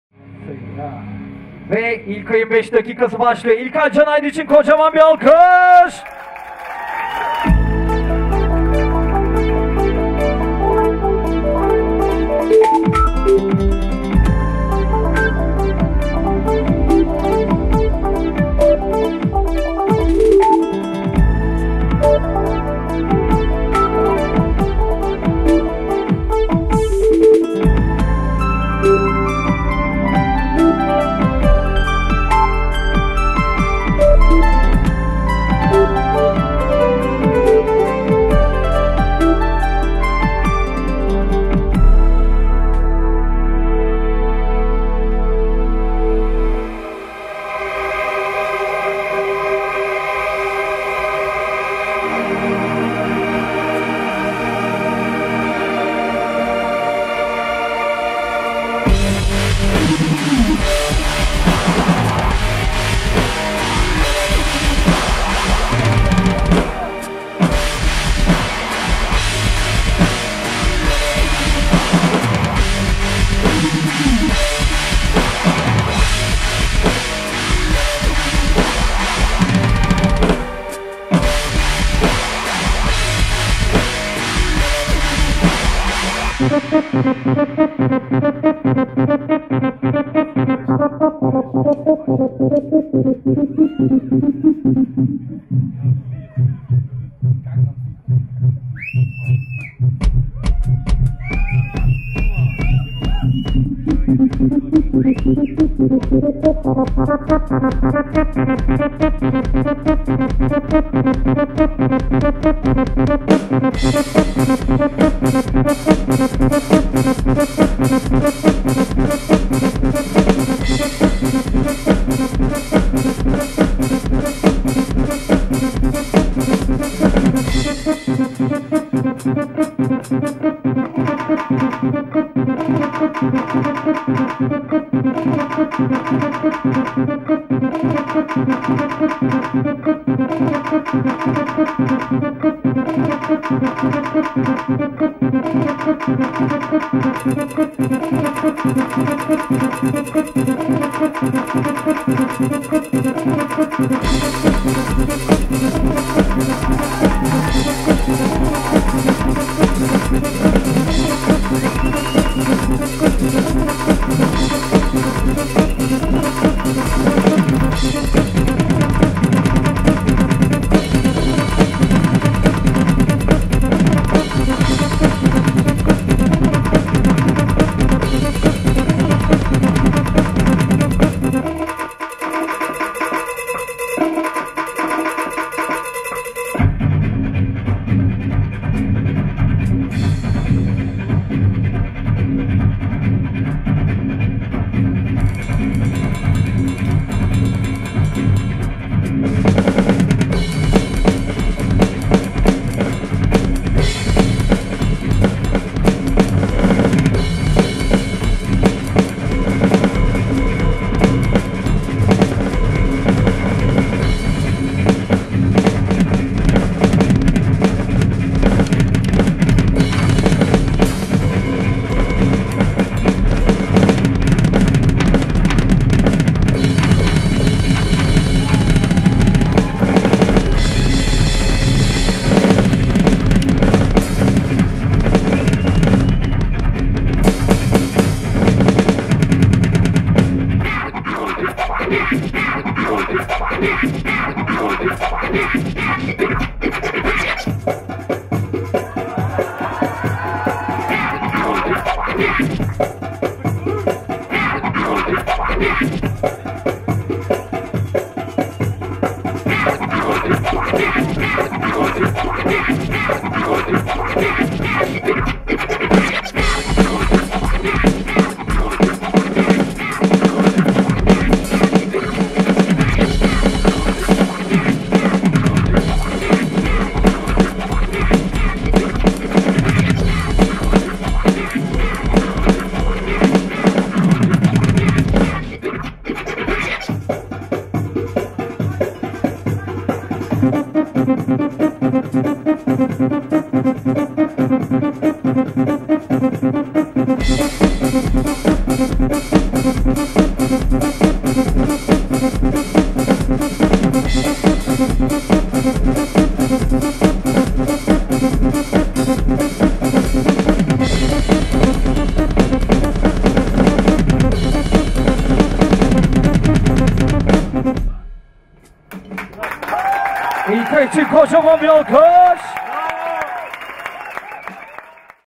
Roland V-Drums World Championship-2 Türkiye - İlkay Canaydın
Finalistlerimizden İlkay Canaydın Performansı..